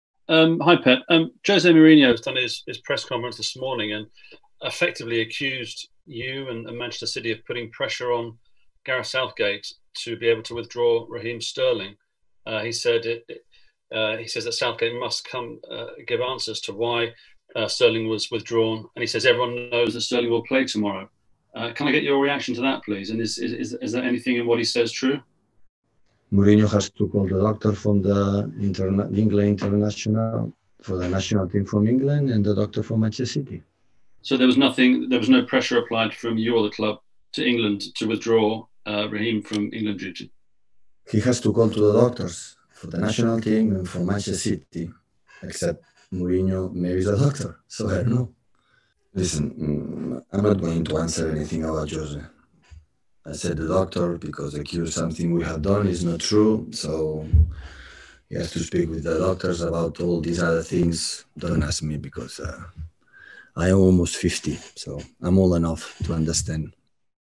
Tottenham - Manchester City | La réplica de Guardiola a la acusación de Mourinho | Diario AS
El técnico del Tottenham acusó al City de presionar a la selección inglesa por Sterling. Guardiola le contestó a su manera en salda de prensa.